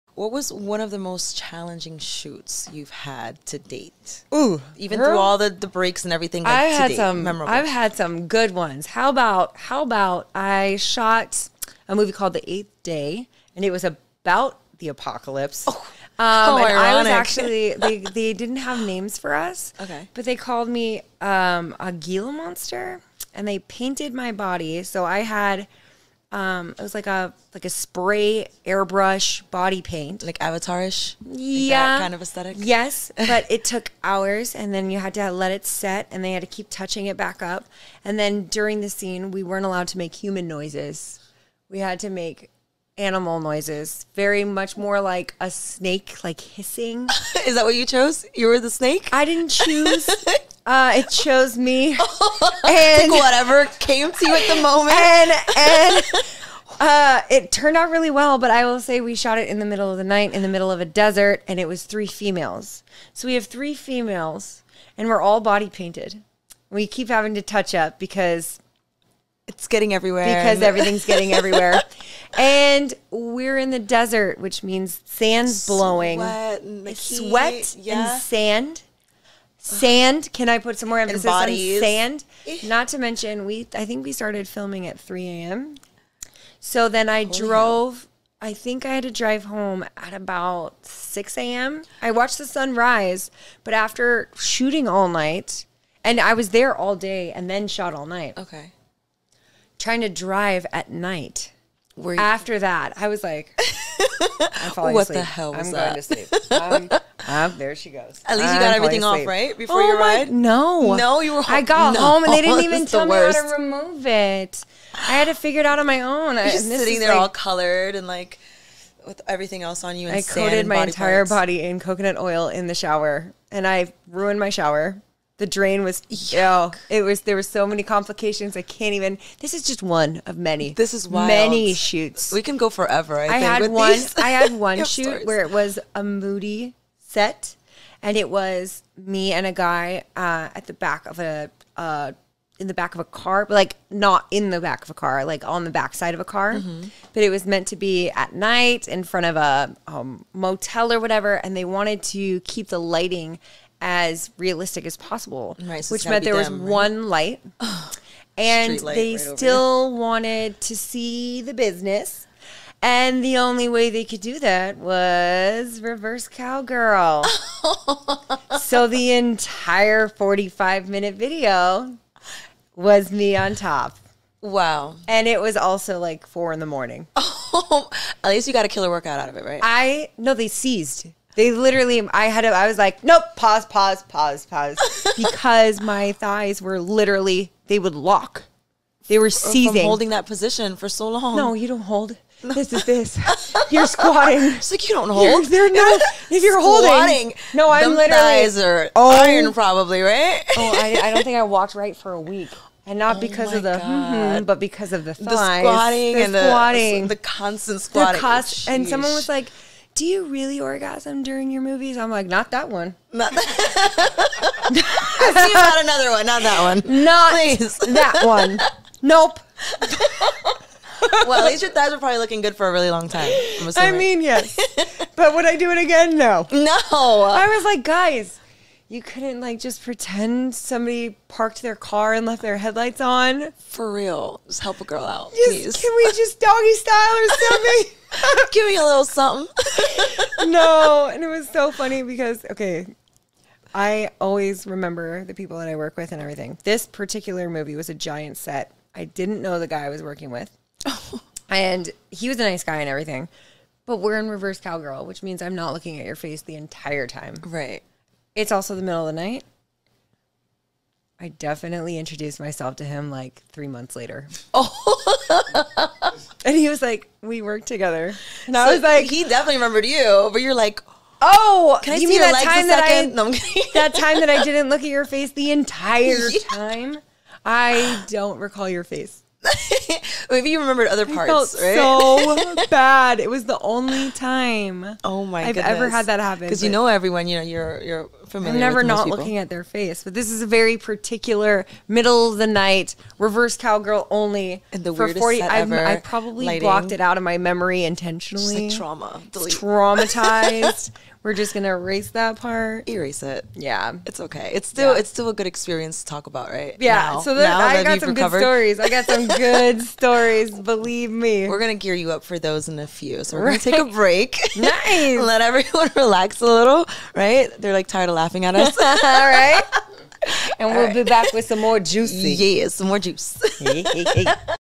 Tori Black - Bad moments on set
Tori goes into detail on one of her worst experiences on set.